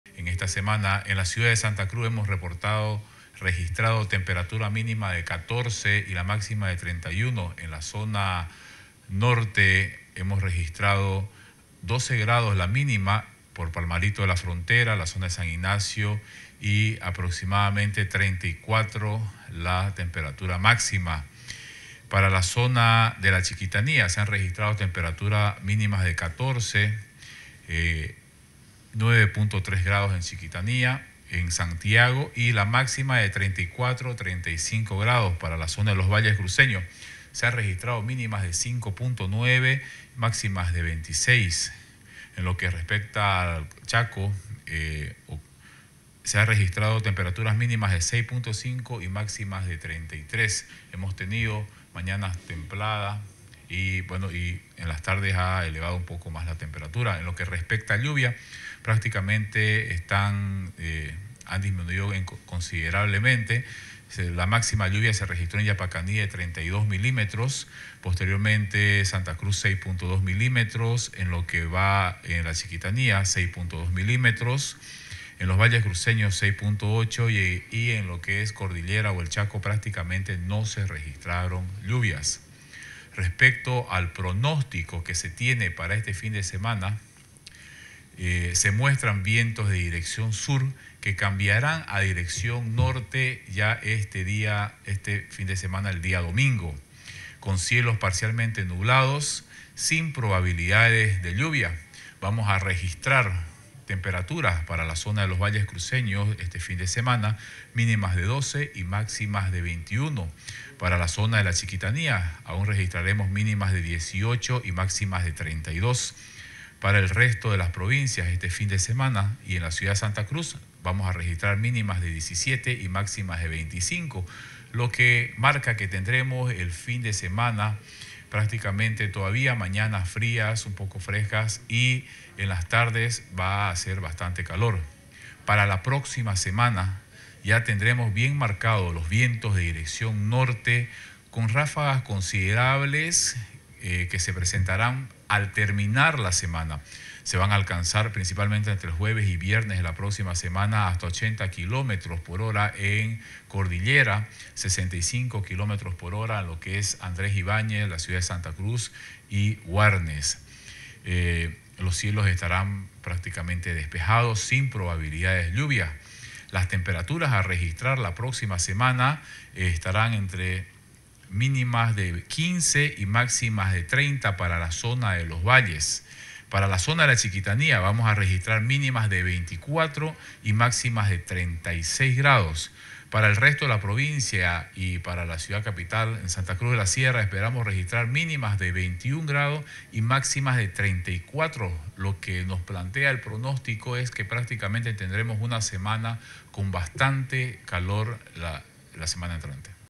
Pronostican clima frío por las mañanas y cálido por las tardes para el fin de semana
El SIA, informó que los vientos de dirección sur cambiarán a dirección norte el día domingo 30 de julio, con cielos parcialmente nublados y sin probabilidad de lluvias.